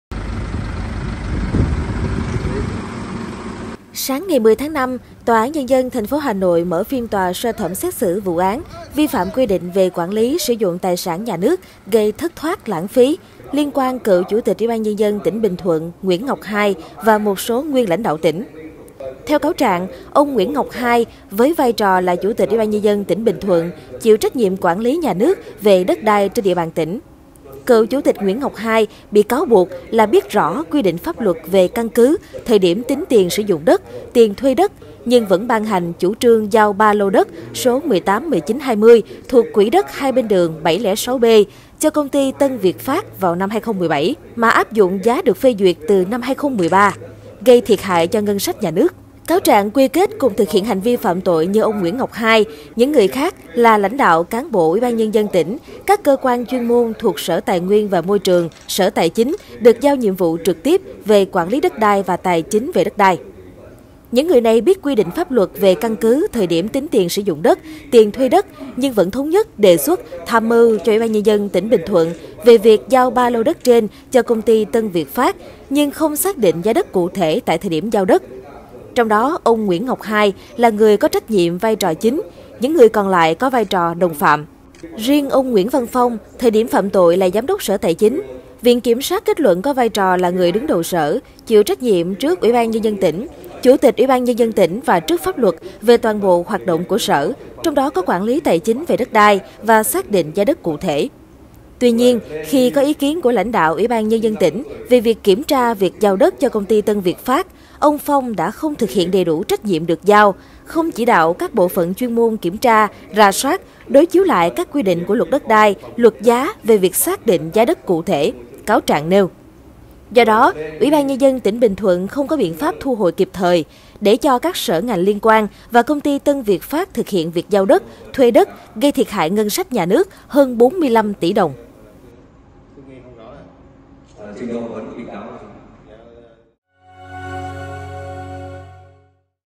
Xét xử cựu chủ tịch Bình Thuận cùng 11 cựu cán bộ trong vụ sai phạm về đất đai
Sáng 10-5, Tòa án nhân dân TP Hà Nội mở phiên tòa sơ thẩm xét xử vụ án "vi phạm quy định về quản lý, sử dụng tài sản nhà nước gây thất thoát, lãng phí" liên quan cựu chủ tịch Bình Thuận Nguyễn Ngọc Hai và một số nguyên lãnh đạo tỉnh.